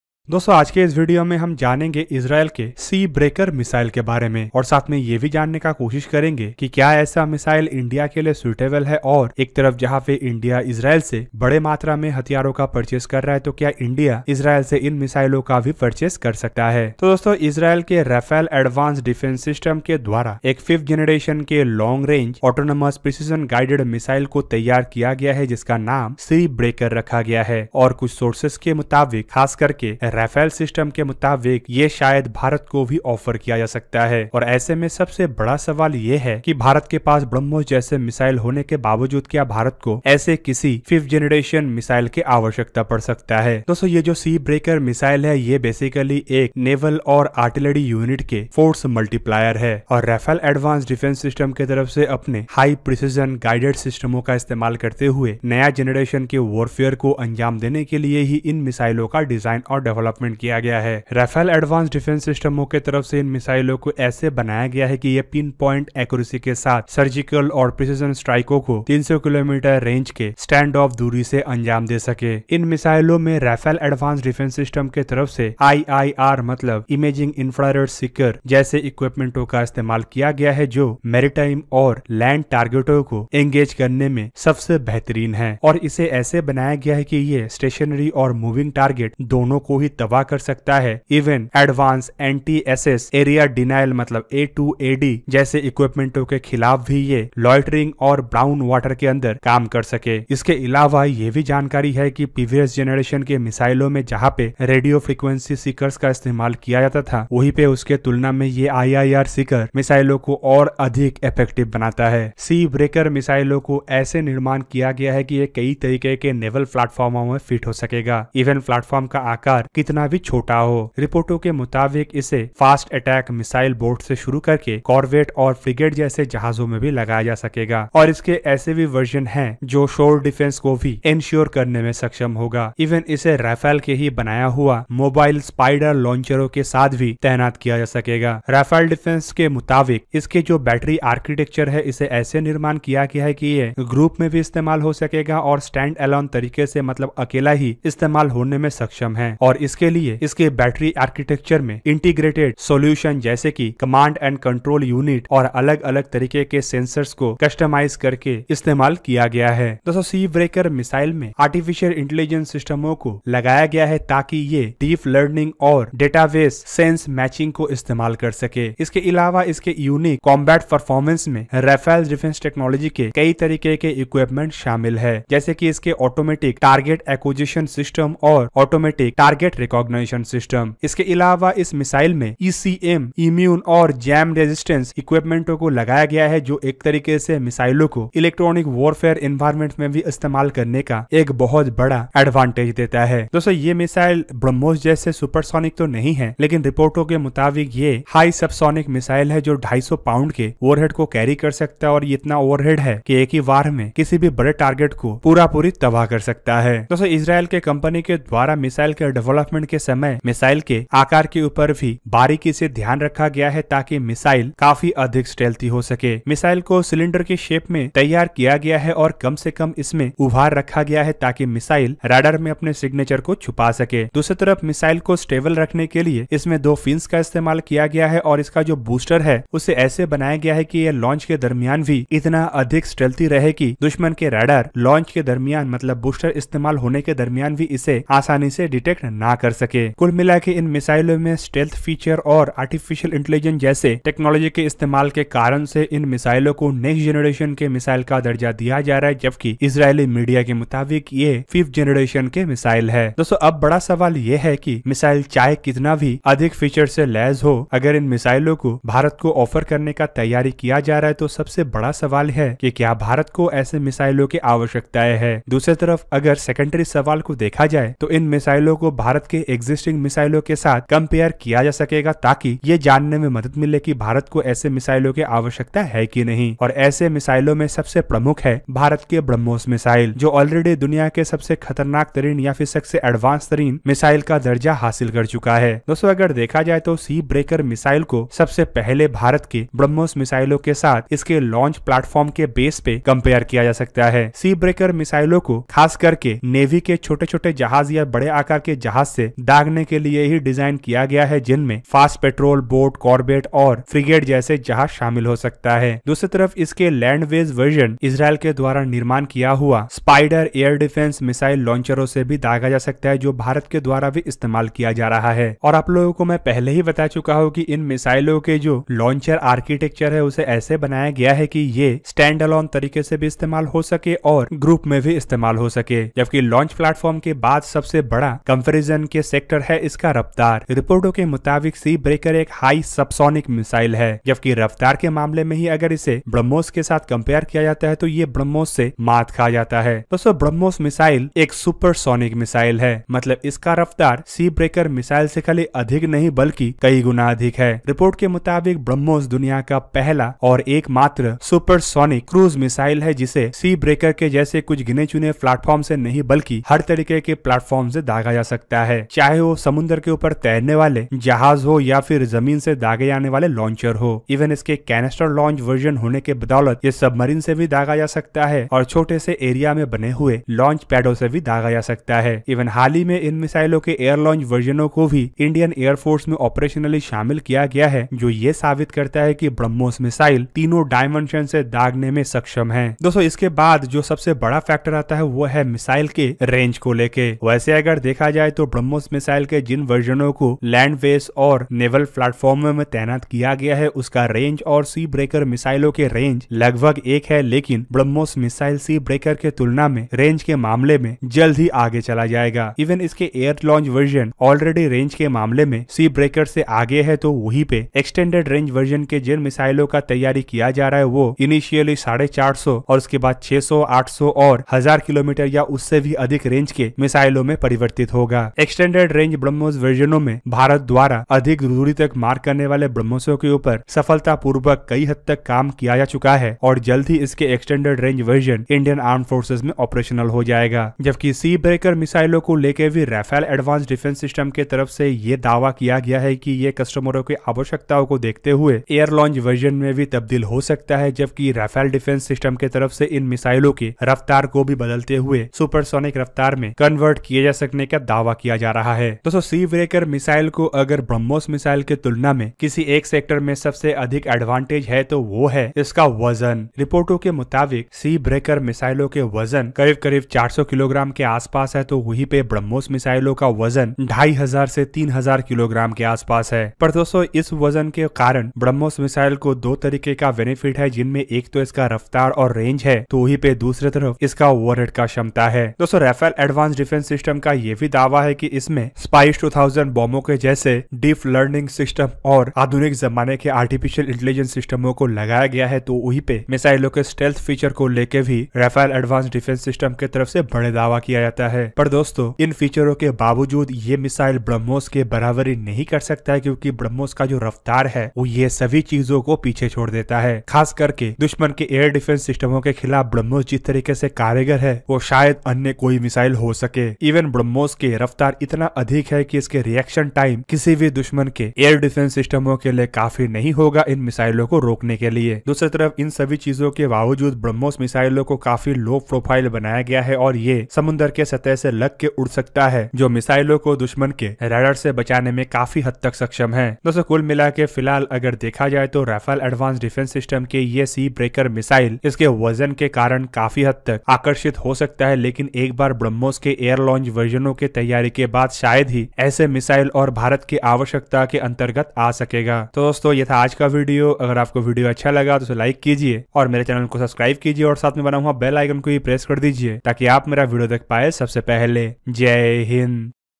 क्या India Israel से Sea Breaker Missile खरीदेगा? Brahmos Vs Sea Breaker
Israel’s Rafael Advanced Defense Systems has introduced a fifth-generation long-range autonomous precision-guided missile system called Sea Breaker and sources indicating Rafael may offer this missile to India, so will India buy Sea Breaker, even so, India already operating indigenous Brahmos missile.

Sea Breaker is a naval and artillery unit force-multiplier, designed to overcome the modern warfare arena challenges, using Rafael’s legacy of high-end precision guided solutions.

Sea Breaker provides surgical, pin-point precision strikes from stand-off ranges of up to 300 km. It features an advanced IIR (Imaging Infra-Red) seeker, ideal for engagement of maritime and land targets, stationary or moving, in advanced Anti Access/Area Denial (A2/AD) arenas, and in littoral or brown water, including archipelago, as well as for engagements in which previous generation RF-seeker-based missiles are not effective.

Sea Breaker can be launched from naval platforms, varying in size, from fast attack missile boats, to corvettes and frigates. The land version is a central part of the shore defense, based on Rafael’s highly mobile SPYDER launchers. The battery architecture supports standalone launchers, or operation as an integrated solution, with a command and control Unit (CCU) and various sensors, based on customer requirements.

Using Artificial Intelligence, Sea Breaker performs deep-learning and big data-based scene-matching, a unique combat-proven Rafael technology, enabling Automatic Target Acquisition (ATA) and Automatic Target Recognition (ATR). The system has full operational capability in GNSS-denied arenas, in all weather conditions. The missile is ECM immune and jam-resilient. Sea Breaker’s mission profile enables sea-skimming and terrain-following low-level flight above ground.

Flying at high subsonic speeds, Sea Breaker has a multi-directional, synchronized full sphere attack capability, based on predefined attack plans, according to waypoints, azimuth, impact angle and aim point selection, ensuring a high probability of mission success, with a 250 lb. penetration, blast and fragmentation, making a single hit effective enough to neutralize a frigate-sized ship.

The missile’s datalink supports real-time man-in-the-loop decision-making and tactical updates. It also features a mid-flight abort capability and Battle Damage Assessment (BDA).

The Missile tail of “Sea Breaker” has a cylindrical shape, which helps to reduce air resistance while reducing radar reflection area; rockets use irregular cruciform rudders; two lift wings spread out, when the rocket leaves the launch pad. The missile’s air intake is designed to be recessed under the missile’s body, to improve its shape and stealth.

In terms of combat block, “Sea Breaker” is equipped with a multi-function penetrating/explosive/fragmentary with a weight of 113 kg, and its destructive power is enough to destroy a frigate.

Rafael’s “Sea Breaker” missile is a multi-domain  that supports a new concept of land and sea; Simultaneously with the sharing of the launcher with the SPYDER missile, this means that the anti-aircraft missile, can be deployed on the same launcher with the cruise missile.

The cruise missile “Sea Breaker”, which will be fitted on Israel’s latest SAAR 6 destroyer, contributes to Israel’s strength and deterrence in the eastern Mediterranean.